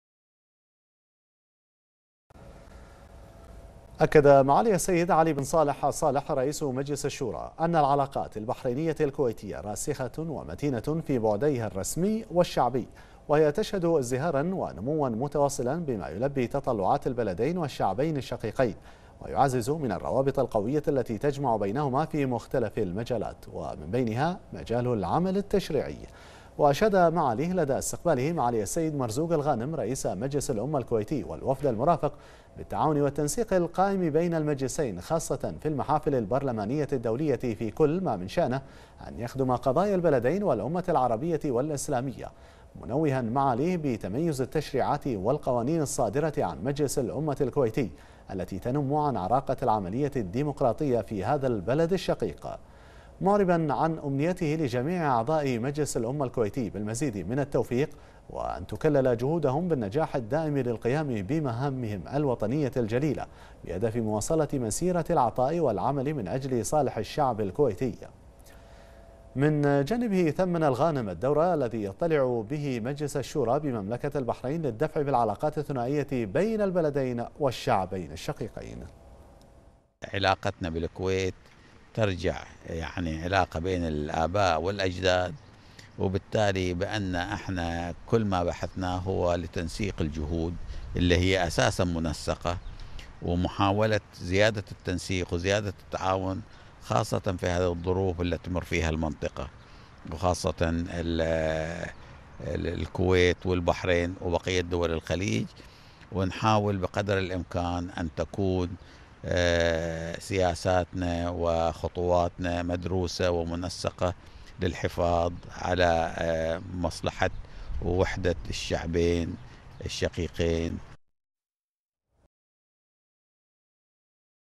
البحرين : الصالح يشيد بتطور العلاقات بين البلدين والغانم يثمن جهود مجلس الشورى لتطويرها
الصالح يشيد بتطور العلاقات بين البلدين والغانم يثمن جهود مجلس الشورى لتطويرها